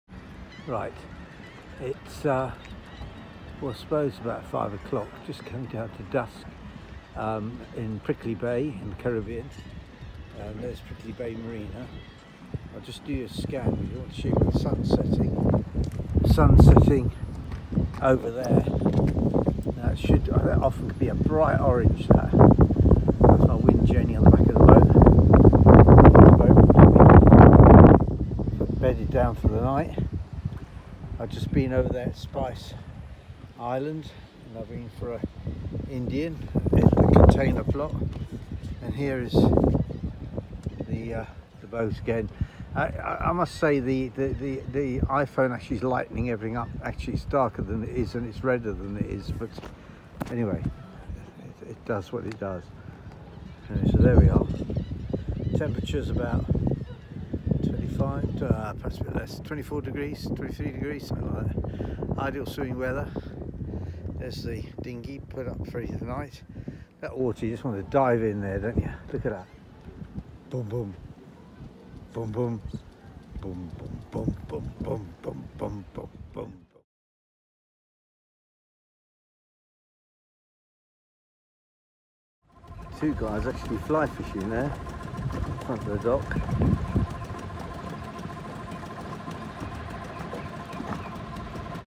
Easter Friday night and Saturday 16/4 .Grenada
As title - fly fishing and sunset